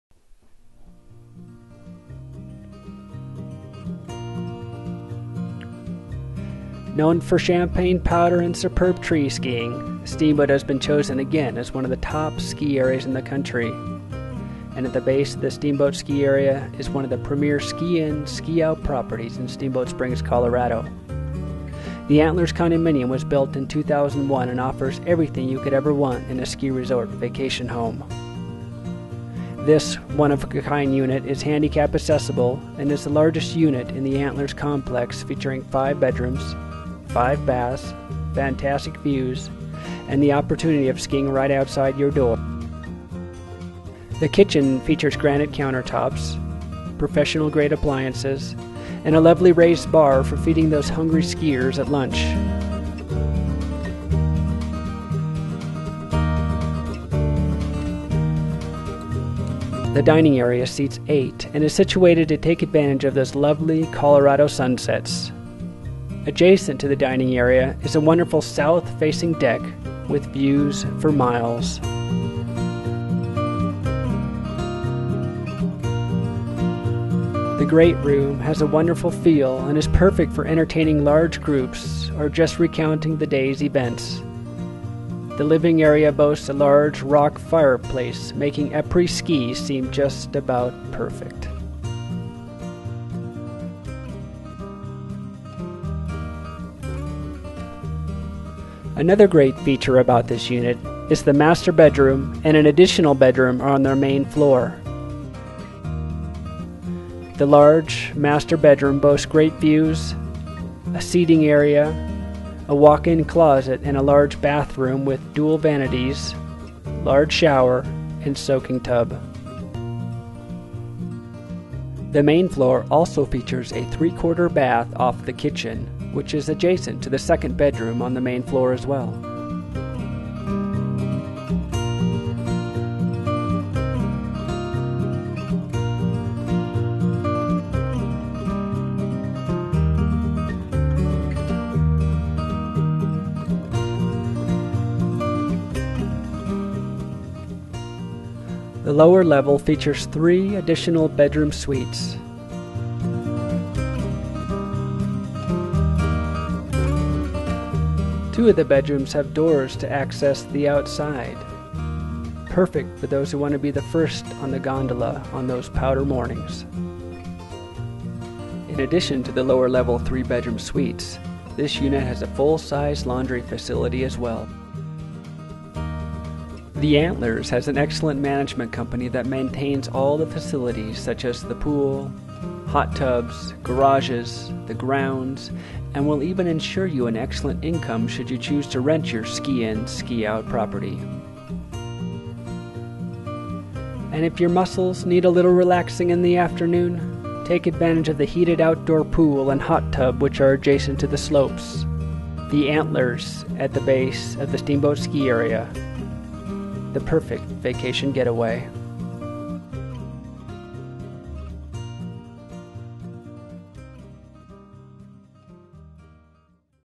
A Premier Ski-in/Ski-out Steamboat Springs Condo
The Antlers at the base of the Steamboat Ski area is the premier ski in/out Steamboat Springs property. This is one of the largest within the Antlers at Christie Base complex. A rarely available true 5 bedroom, 5 bath luxury ski area residence boast 2,641 square feet of sure elegance. Enjoy the comforts of living slope-side to one of the top rated ski areas in the country. Champagne powder awaits . . right outside your door. With so few true ski in/ski out properties in Steamboat, this is an opportunity. Fabulous views of the ski runs and down valley from nearly every room in the place. The kitchen has stainless steel appliances, granite countertops, and great seating bar for those fast meals on the go. The master suite and an additional bedroom are both on the main level with 3 additional bedrooms are on the lower level. It's also one of the only wheel-chairs accessible units in the complex with no steps into or through the main level. It's also perfect for large families or groups. Every bedroom has its own bathroom and there's plenty of room for ski gear with all the expanded storage available. All the conveniences one expects in the ultimate family retreat. Exceptional property management offers outstanding owner amenities including on-site call shuttle service, on-site concierge, front desk check-in, private owners ski locker complete with lounge, steam shower and dressing rooms, ski valet, heated walk-ways and slope-side swimming pool with outdoor hot tub. No need for shuttles, parking, or even walking, ski right out your door to be first up the gondola. Paradise awaits. And this unit is sold fully-furnished by one of Steamboat's finest interior decorators.